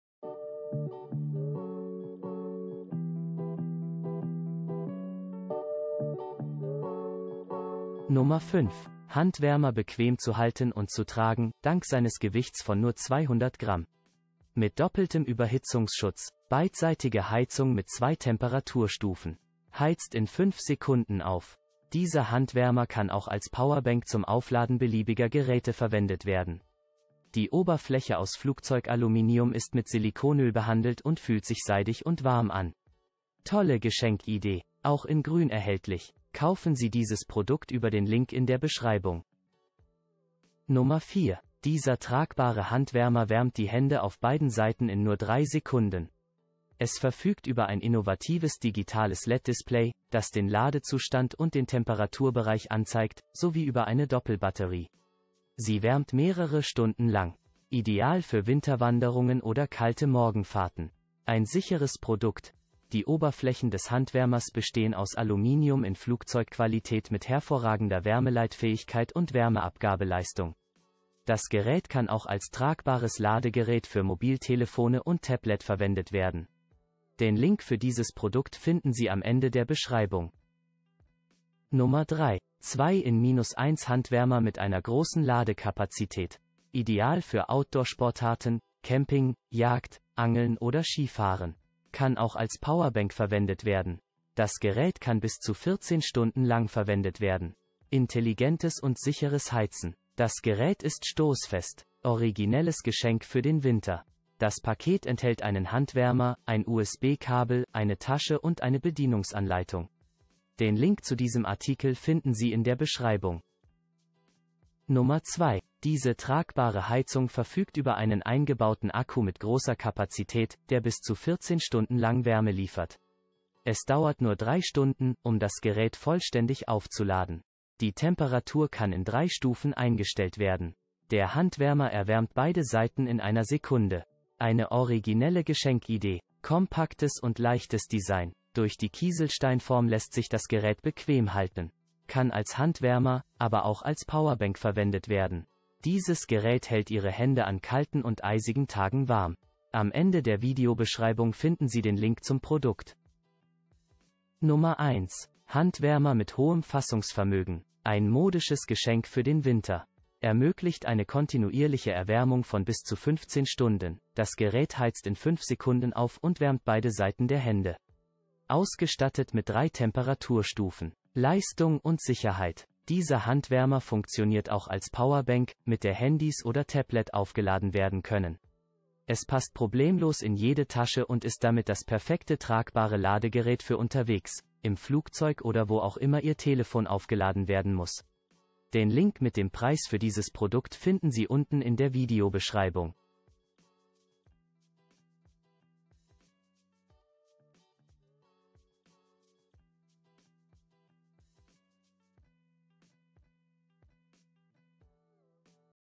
Die 5 besten wiederaufladbaren Handwärmer mit Powerbank
✋ Hier ist die 5 besten wiederaufladbaren handwärmer mit powerbank, auf dem Markt verfügbar .

 5) WATMHHJQ Handwärmer Wiederaufladbare USB 10000mAh Tragbare H...

 4) Unigear Handwärmer wiederaufladbar, 5200mAh Wiederverwendbar...

 3) VOONEEN Handwärmer PowerBank, 2 in 1 Elektrisch Handwärmer 9...

 2) Awroutdoor Handwärmer Schnellladung USB, 9000mAh PD wiederau...

 1) OCOOPA 10000mAh Handwärmer, Wiederaufladbar Elektrische Trag...

Zeitstempel (Timestamps)

00:00 - Einführung
00:08 - 5) WATMHHJQ Handwärmer Wiederaufladbare USB 10000mAh Tragbare H...
00:40 - 4) Unigear Handwärmer wiederaufladbar, 5200mAh Wiederverwendbar...
01:21 - 3) VOONEEN Handwärmer PowerBank, 2 in 1 Elektrisch Handwärmer 9...
01:54 - 2) Awroutdoor Handwärmer Schnellladung USB, 9000mAh PD wiederau...
02:34 - 1) OCOOPA 10000mAh Handwärmer, Wiederaufladbar Elektrische Trag...